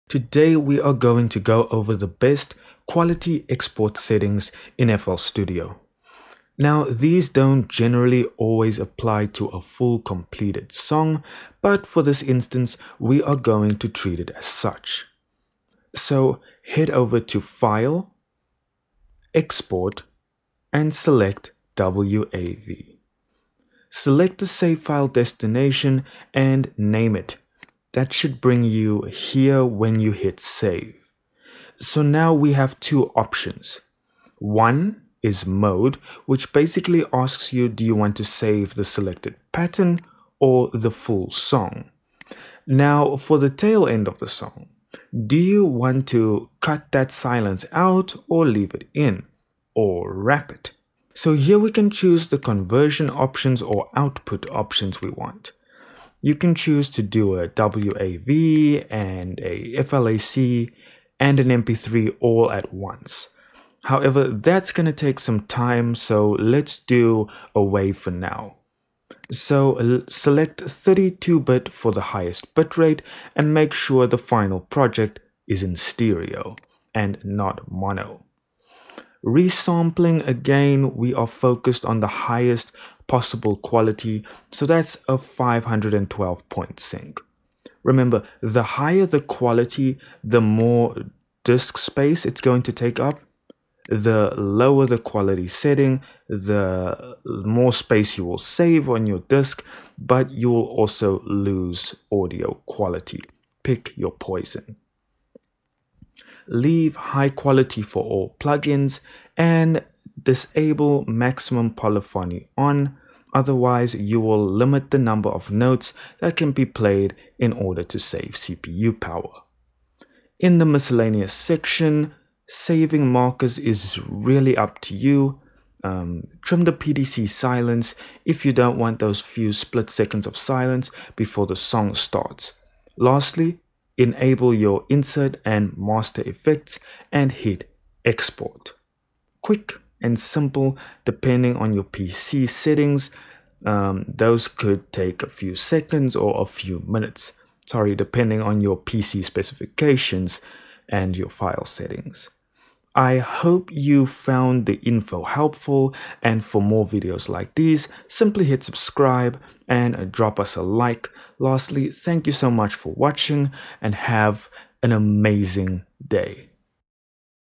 Best Export Settings FL Studio 21 (2026)
In this fl studio tutorial i show you the best export settings for fl studio 21.